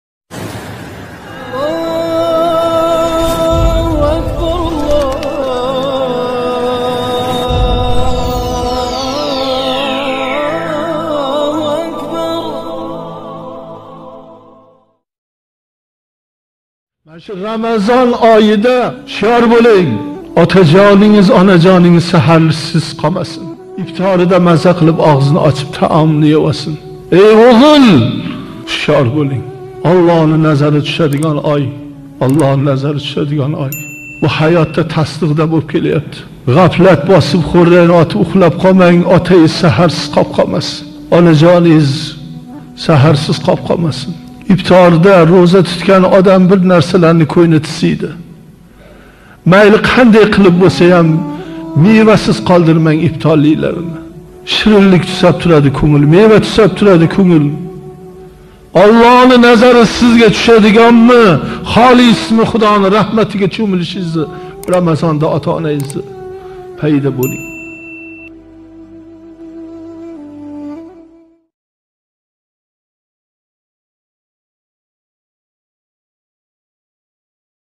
Fozil Qori | Ramazon Oyida Ota-Onangizni Hizmatida Bo'ling ☝️
Assalomu aleykum azizlar ota onalarimiz hamisha baxtimizga doim sog' salomat bo'lishsin amin. 😊